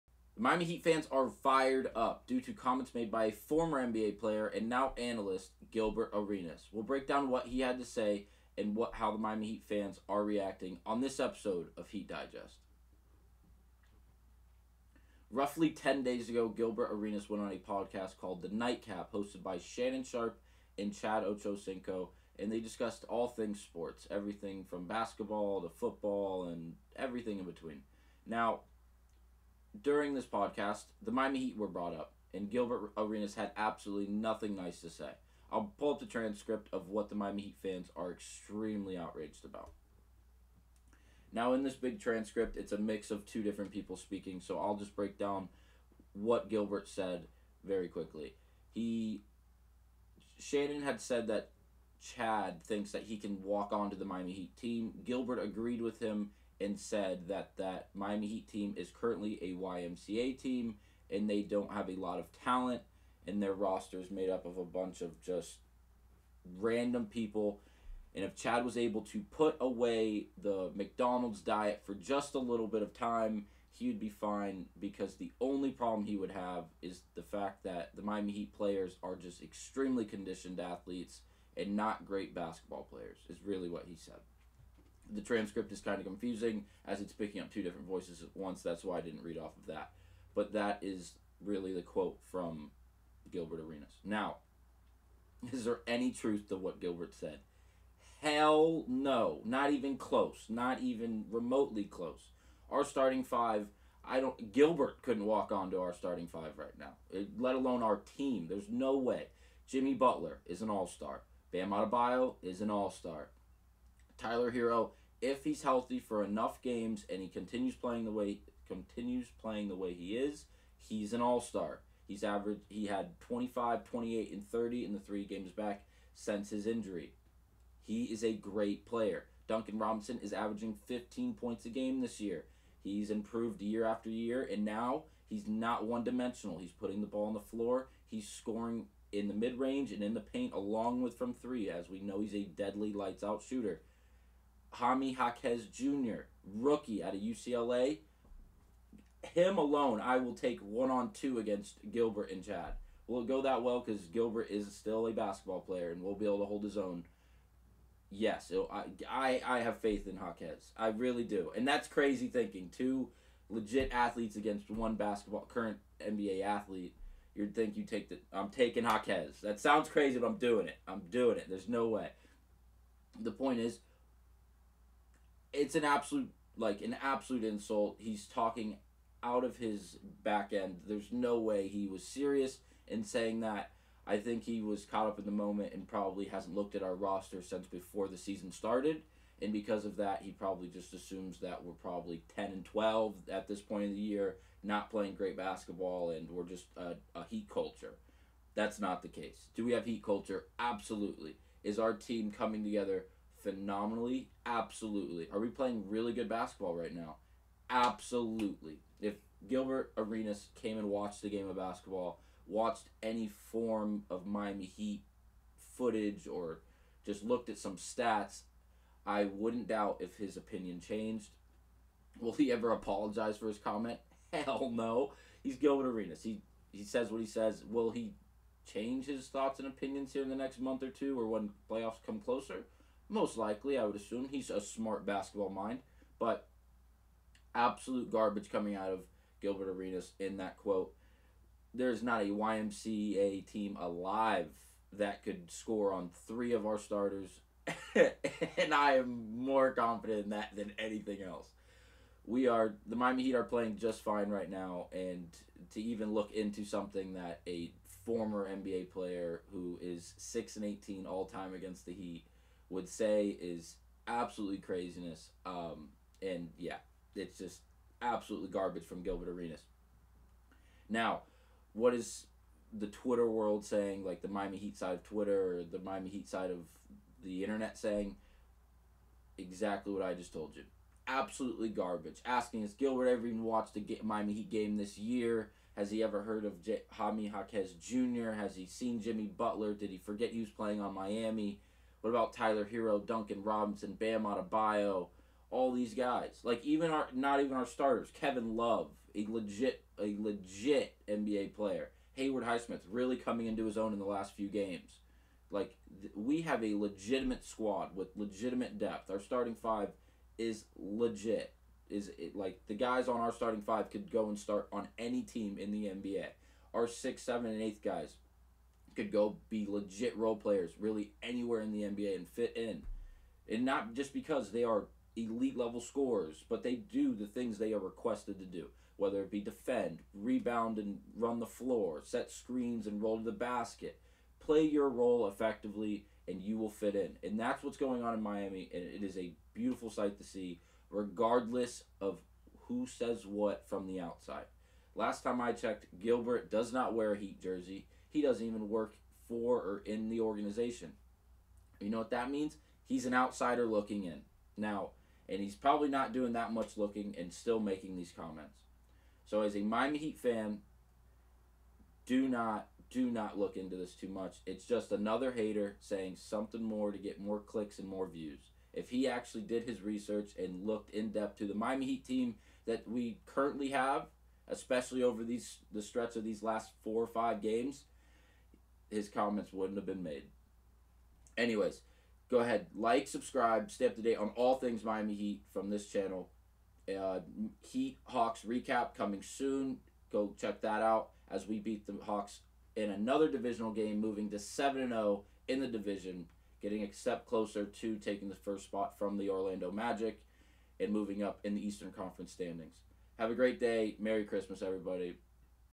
This has Heat Fans FIRED UP...
Houston Kellman breaks down an insulting quote from Gilbert Arenas about the Miami Heat and how the rest of the Miami Heat Nation is reacting to it.

0:00 Miami Heat News
0:15 Heat Fans Are FIRED UP
0:40 Break down of Arenas quote
1:50 Heat roster 'review'
6:25 Closing Remarks